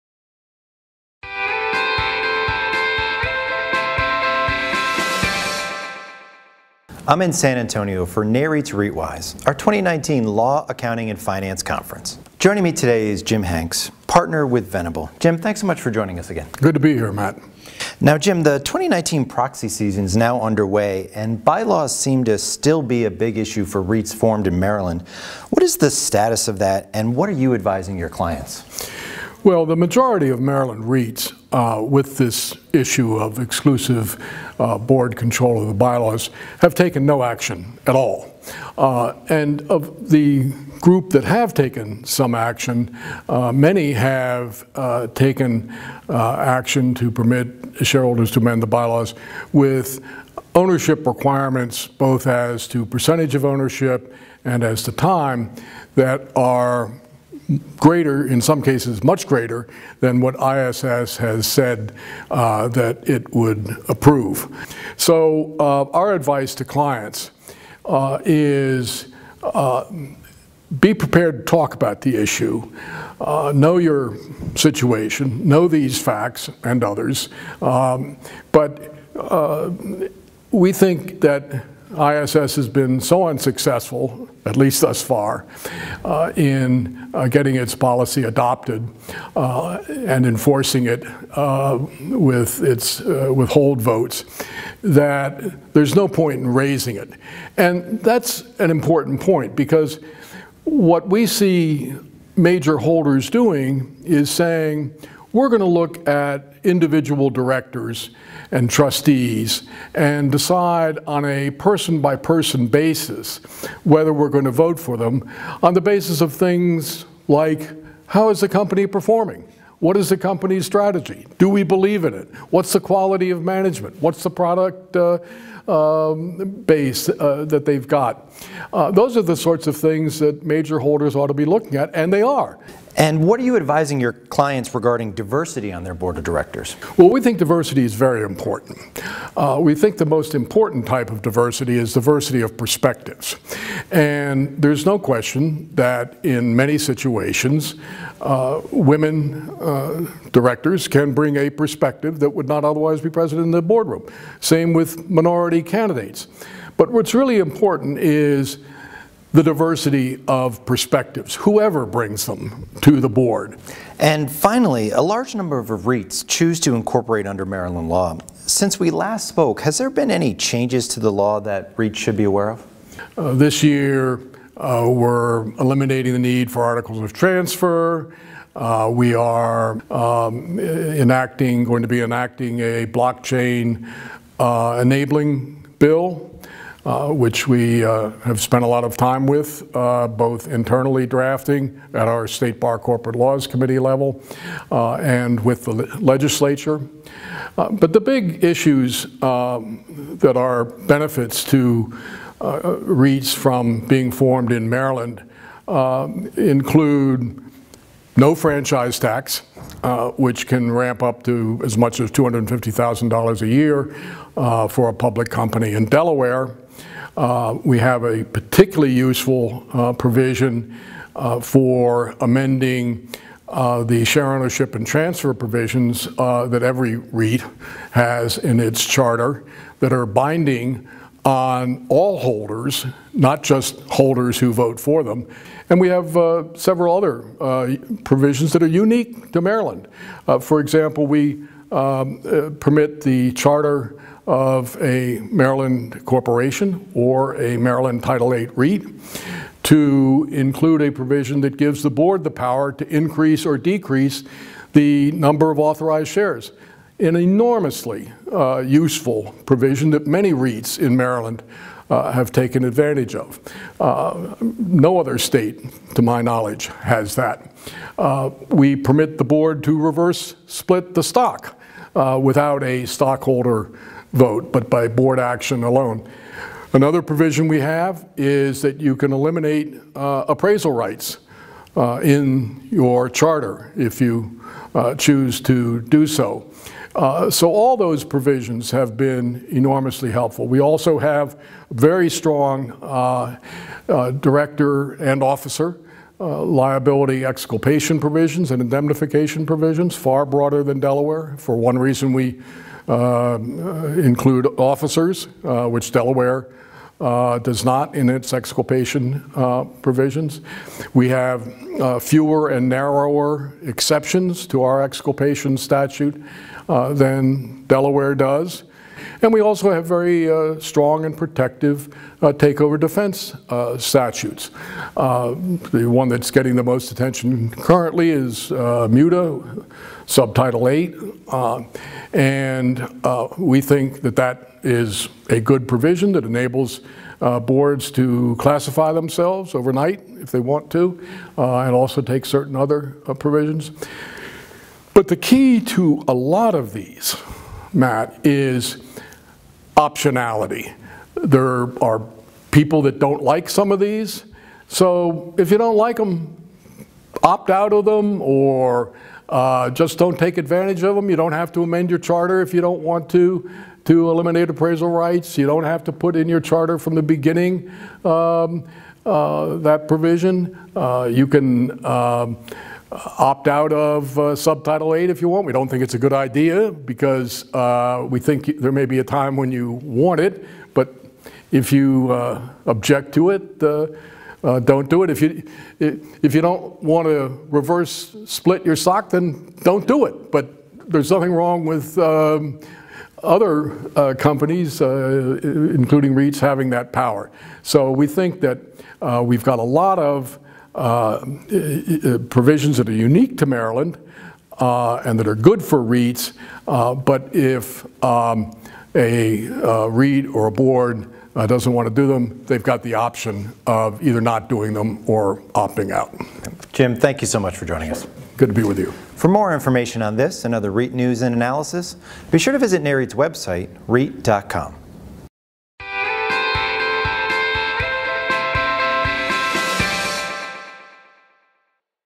Governance Expert Highlights Importance of Diversity of Perspectives
Venable’s Jim Hanks says women and minorities bring unique perspective.